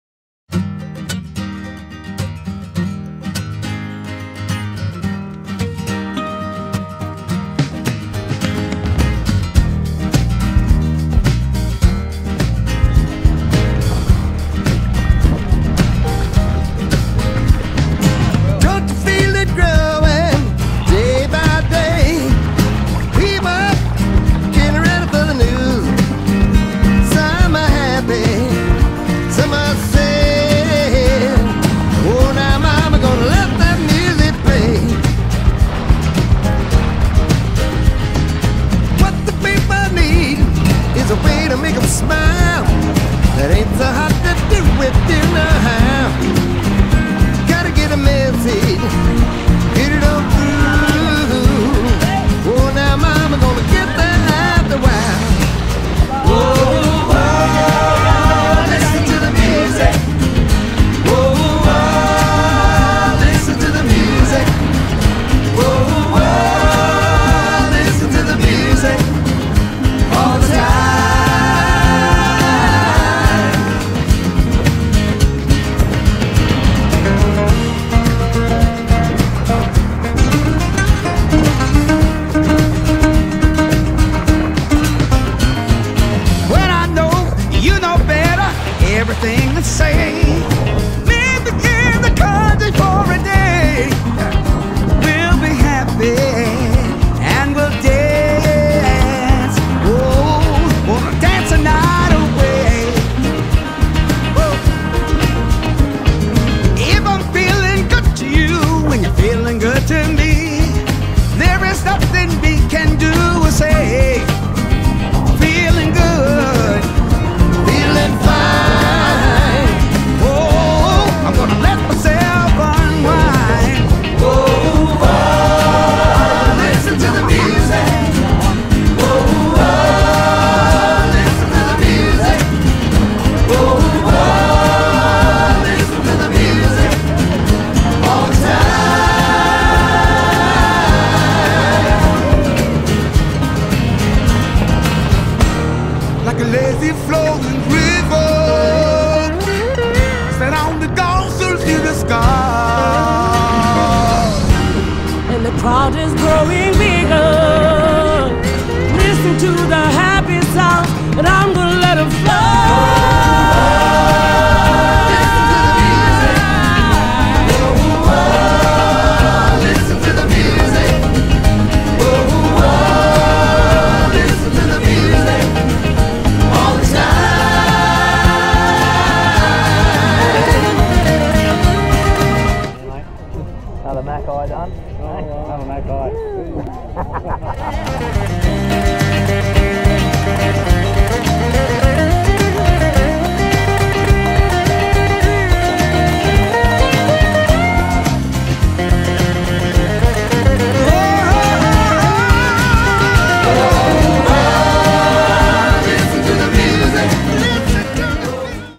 Makai Cup 2023
A classic Makai Cup, downwind from Sussex Inlet to Ulladulla on the pristine NSW South Coast, in a stiff nor' easter than built and built as we got further down the course.
It's hard to imagine a more beautiful, or better run ocean race anywhere in the world.